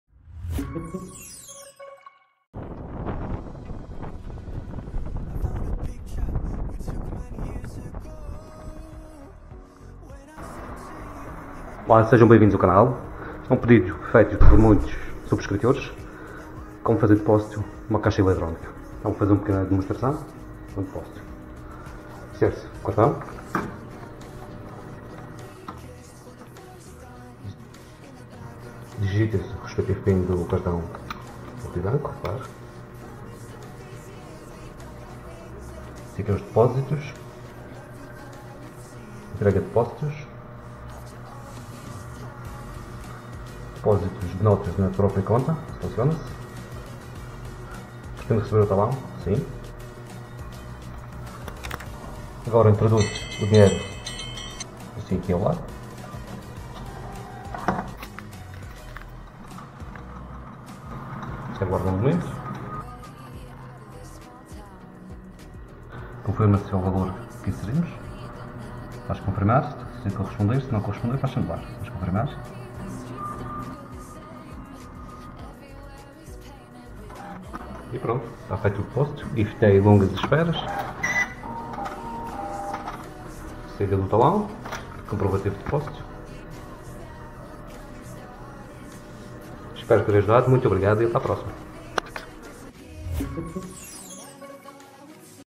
Como depositar dinheiro na conta bancária em Portugal
Deposito bancário, saiba como usar o multibanco.
No video de hoje, explico como usar o caixa eletrónico para fazer deposito bancário.